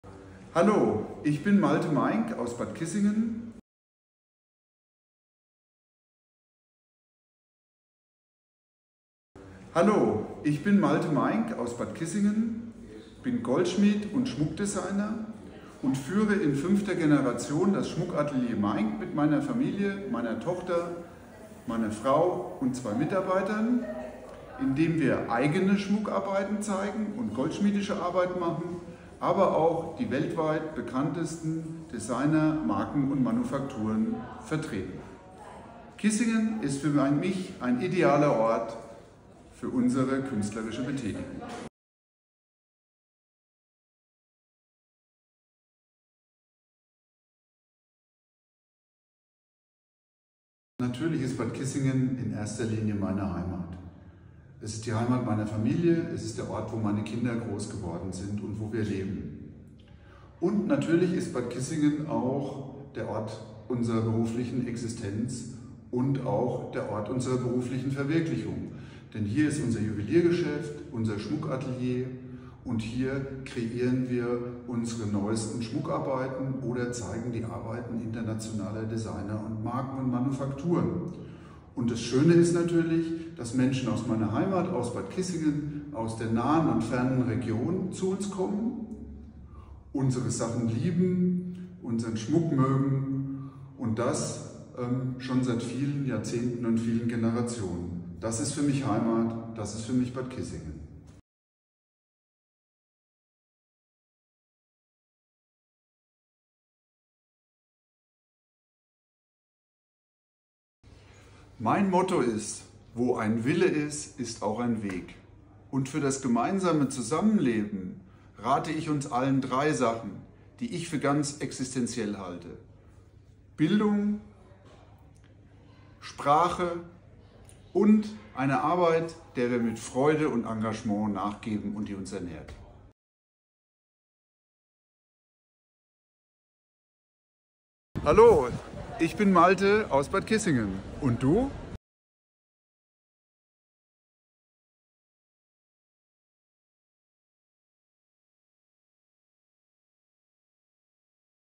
Malte stellt sich vor – Integration in Bad Kissingen
Wir stellen Euch im Rahmen unserer Integrationskampagne Menschen vor, die aus anderen Ländern zugewandert sind, die sich beispielhaft in unsere Stadtgesellschaft in Bad Kissingen integriert haben und sich für die Gemeinschaft engagieren.
Der Erfolg unserer Kampagne hängt auch davon ab, dass Menschen mitmachen und helfen, die immer schon hier leben. Heute lernt ihr Malte kennen, der mit ihrer Familie in Bad Kissingen lebt. In fünfter Generation führt er das Schmuckatelier Meinck.

"WirAlleSindBadKissingen - Zusammen reden. Zusammen arbeiten. Zusammen arbeiten."